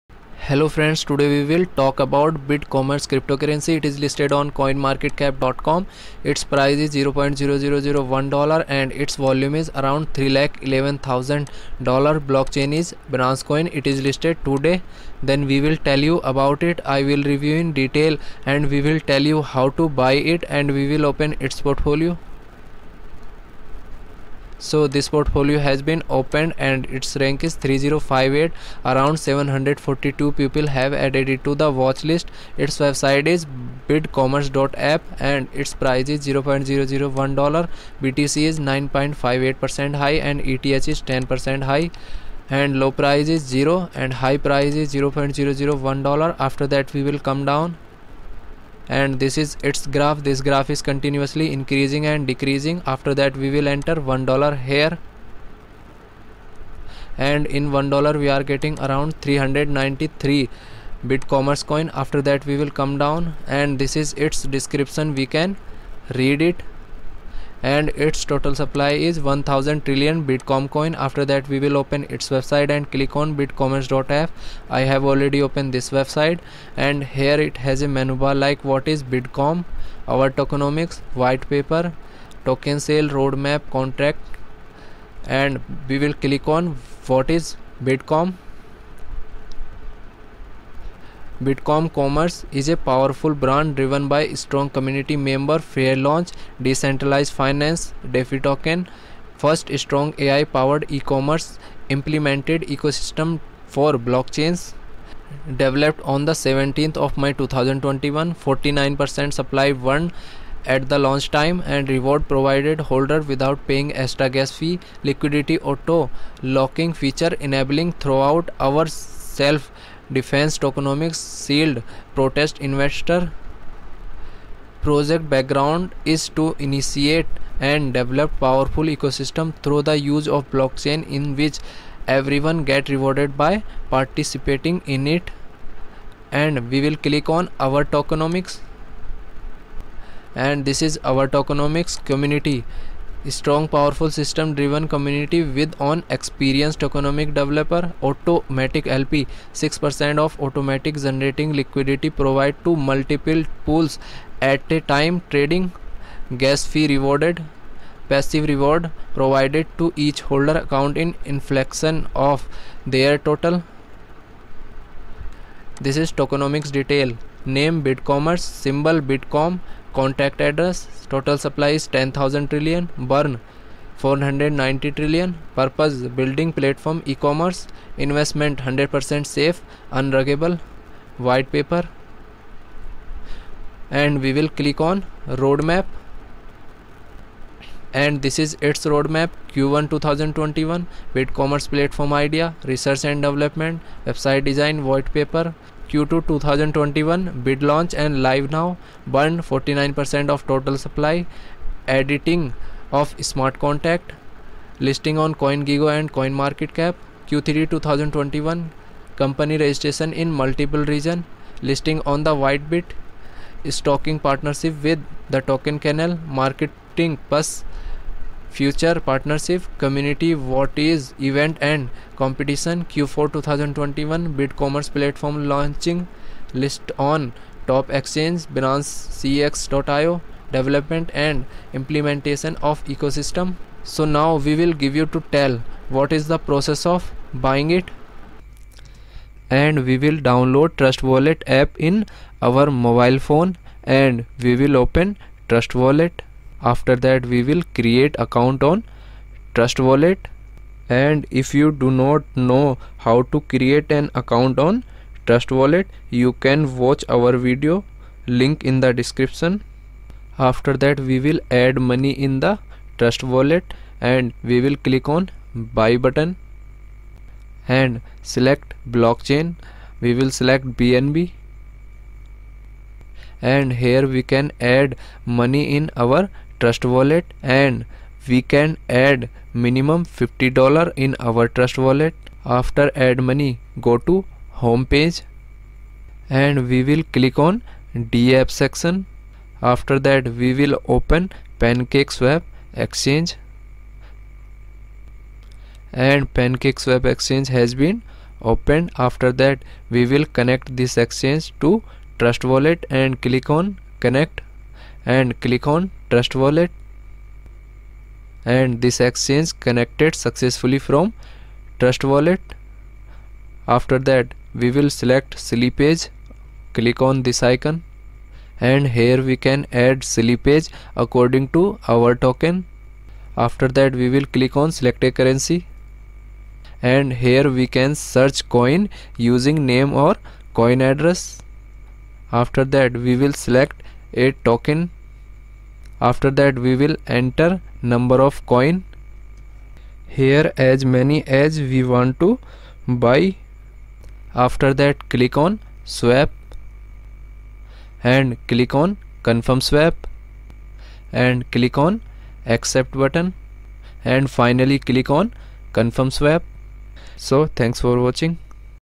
Bidcommerce Token Review in Details and how to buy | Bidcommerce Coin |Bidcommerce Crypto 💰 Currency
How to buy and view detail review of Bidcommerce Coin and The BidCommerce (BidCom) is a Powerful Brand driven by Strong Community members, Fair launch Decentralized Finance (DeFi) Token. First Strong AI-Powered e-Commerce implemented Ecosystem based on Web3.0 for Blockchains.